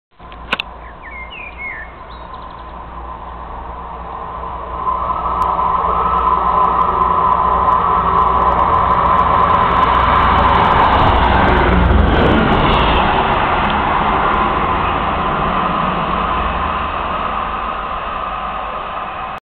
"boring driver day" with 158863 @ west allerton 12/8/2011
the last of the boring drivers at west allerton